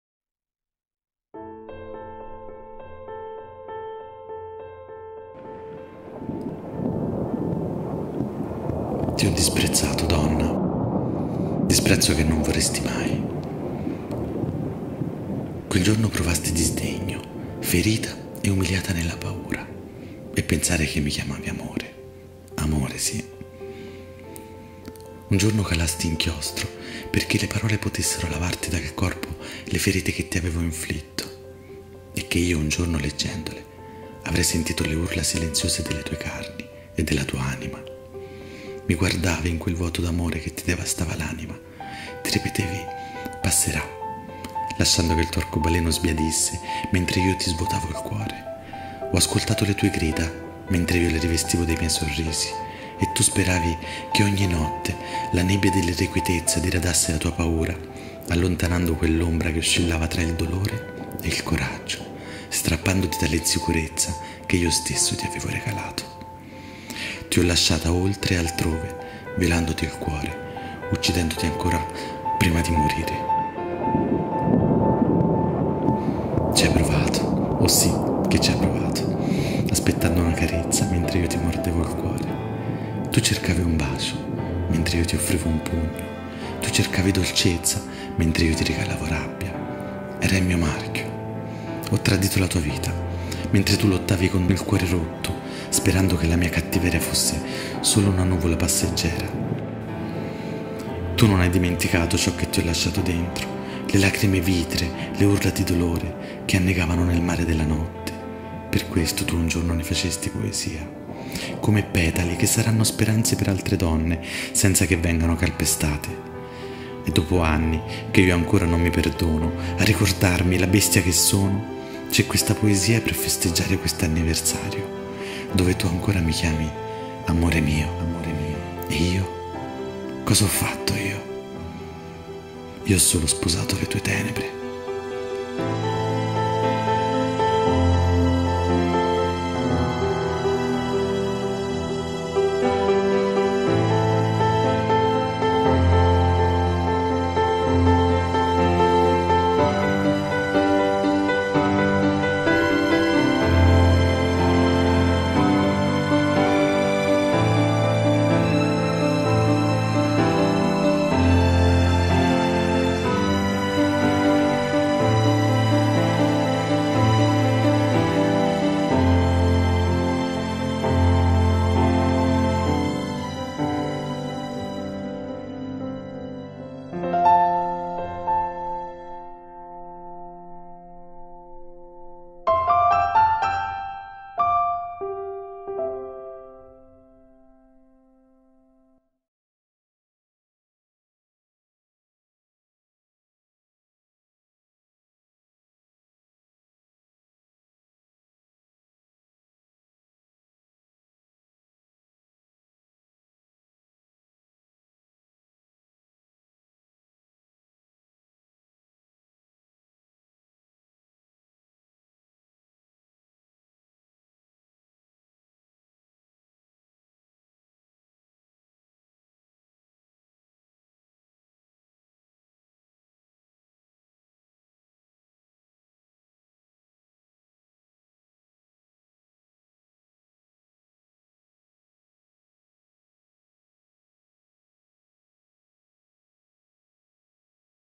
Femminicidio " Tu. Una Donna, Tante Storie"
Poesia scritta dal Gruppo Facebook "Oltre le nostre emozioni" Ideato da Asianne Merisi. Interpretata da Dado Martino